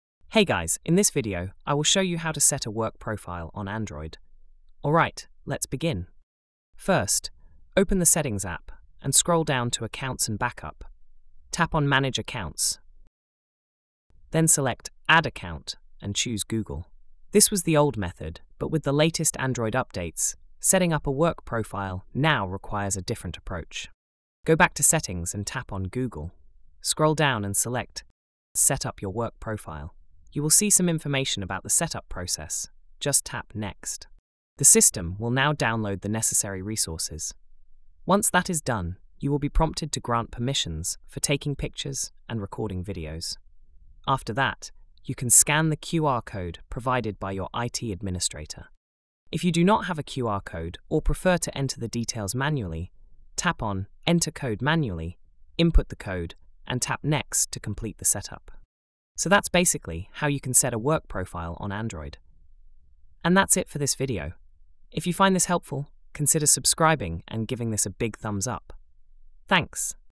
How To Setup Google Work Profile On Android
In this video, I’ll show you how to set up a Google Work Profile on your Android device. A work profile keeps your personal and work apps separate, making it easier to manage your work-related tasks securely. I’ll guide you through each step to get your Google Work Profile set up quickly and easily.